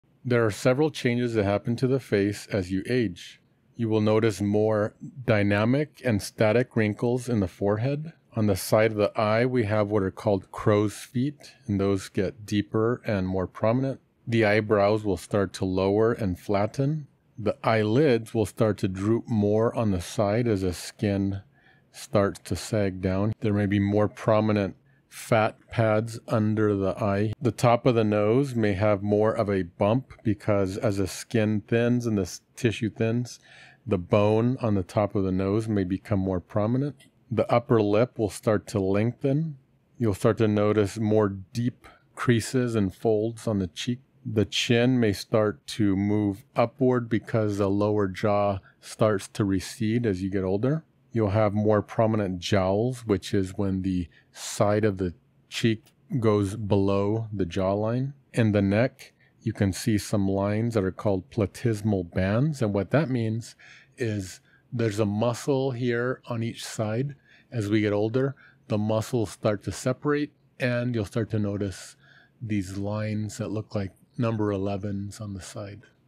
How Faces Age
Dr. Camacho describes how faces change as we age.

The views expressed in this video are those of the author(s) and do not reflect the official policy or position of the Department of the Army, Department of Defense, or the US Government.